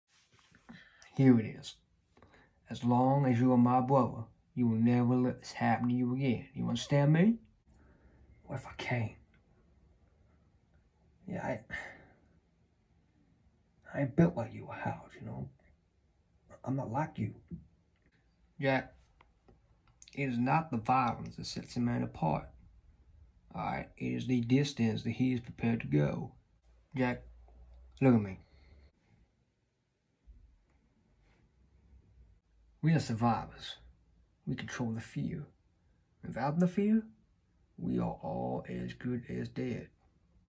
Showreel 1/lawless
Starting an acting journey, so I thought I’d start with a scene from one of my favourite films in lawless

Director: John Hillcoat
Starring: Tom Hardy, Shia LaBeouf, Gary Oldman